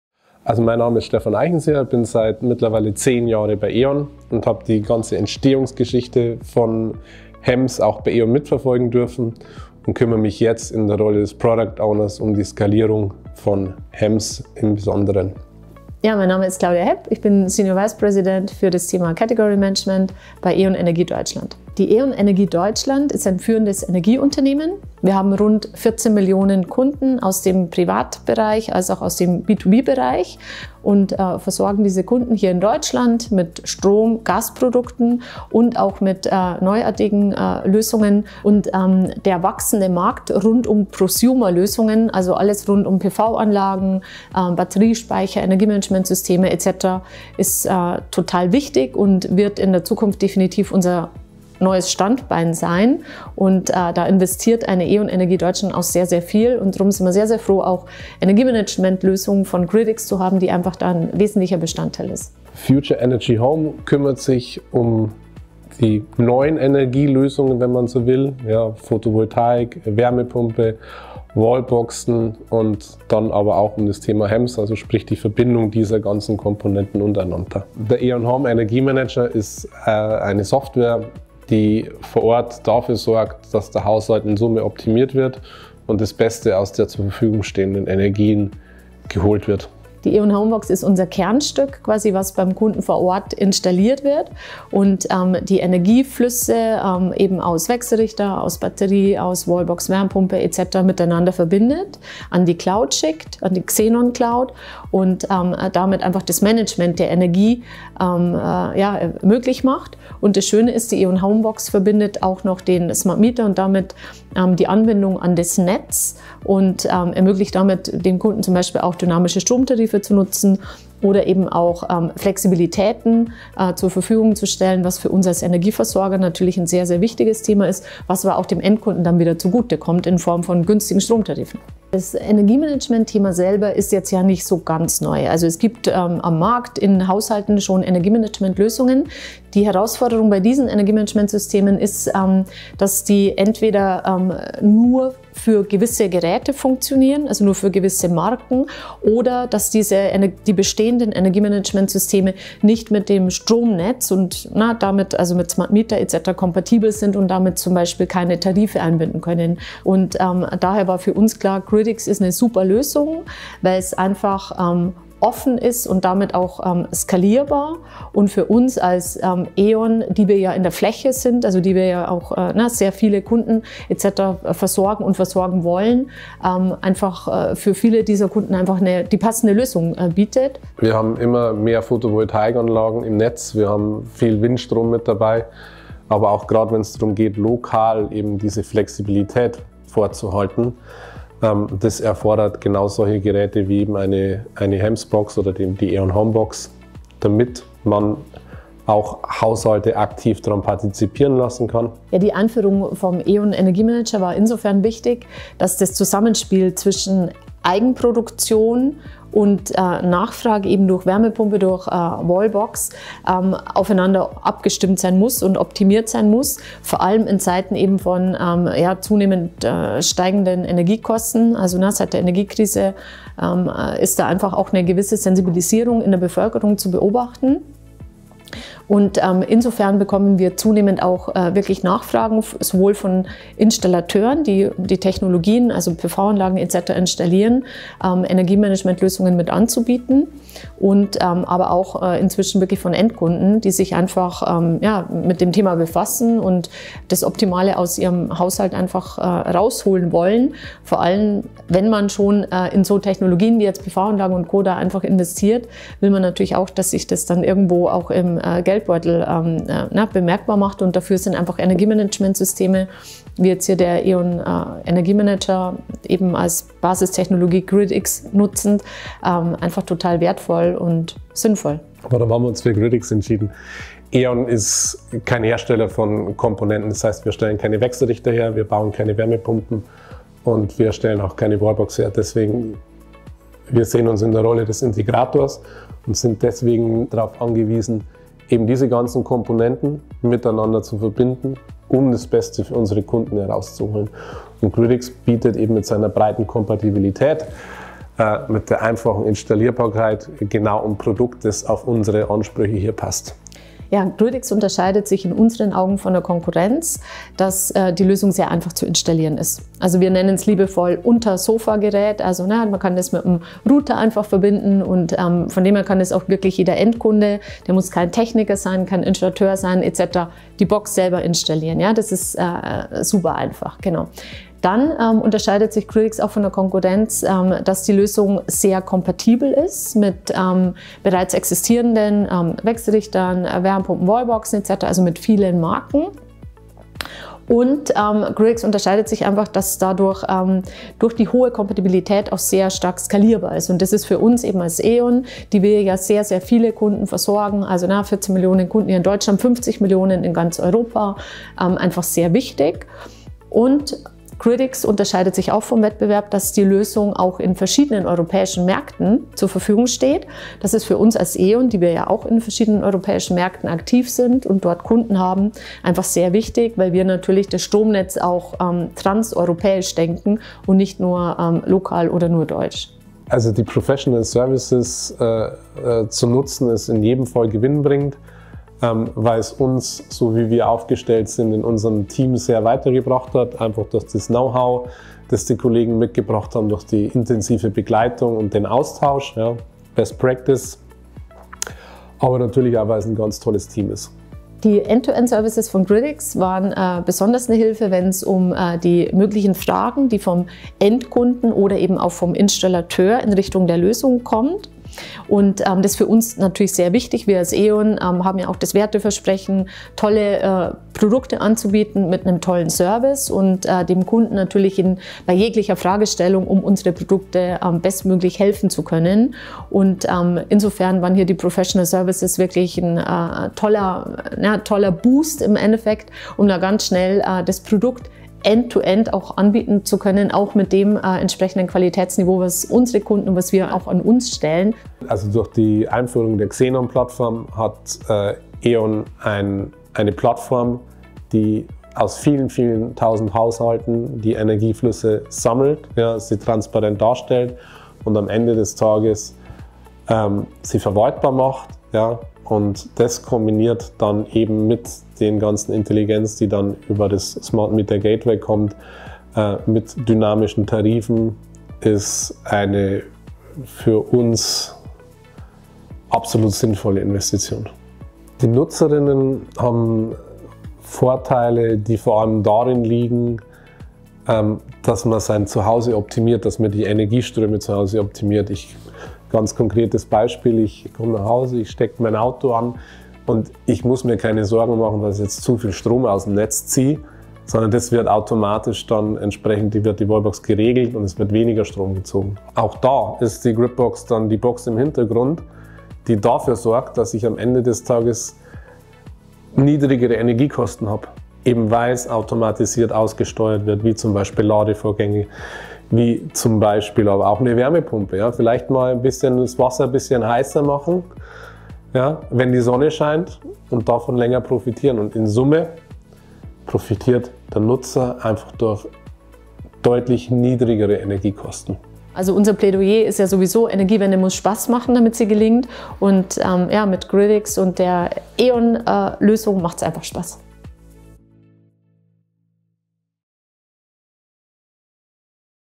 E.ON Energie Deutschland's Success Story: Scaling smart energy management solutions with gridX
E.ON partnered with gridX to launch its own smart energy management solution. Learn more about the partnership as Claudia Häpp, Senior Vice President Category Management at E.ON Energie Deutschland, and Stefan Eichenseer, Product Owner HEMS at E.ON Energie Deutschland, share their personal insights into E.ON's swift rollout of their fully scalable solution, E.ON Home Energiemanager.